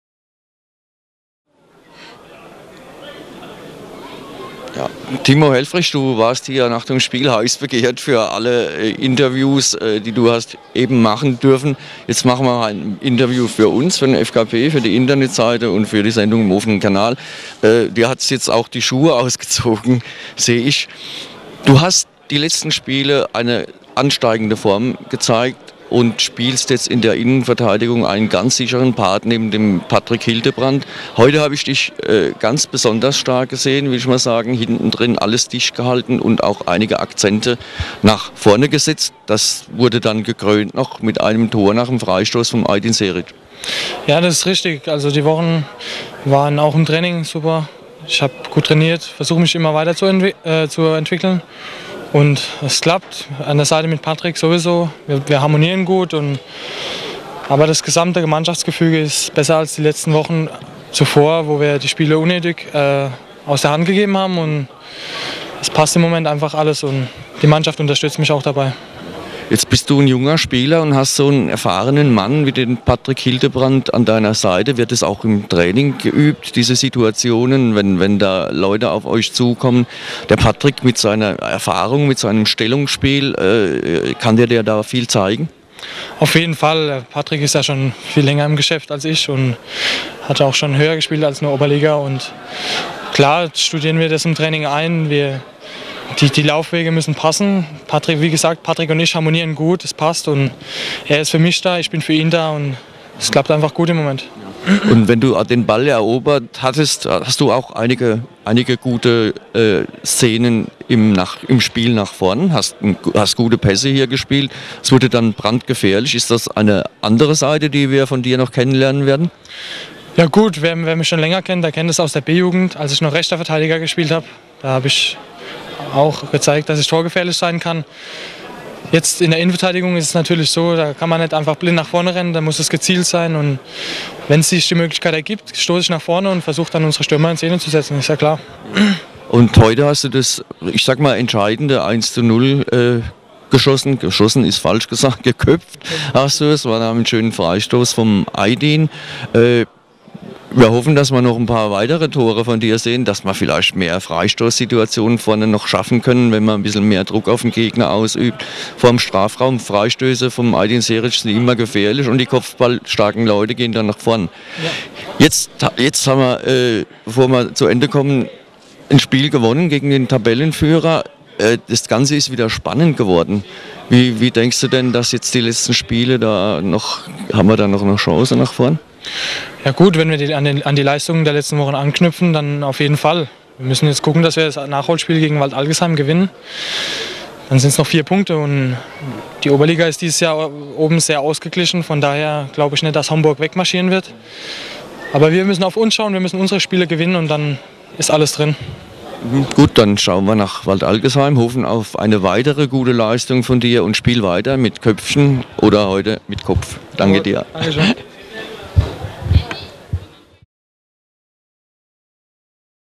Interview mit Timo Helfrich
Interview mit FKP-Spieler Timo Helfrich / FK Pirmasens - FC Homburg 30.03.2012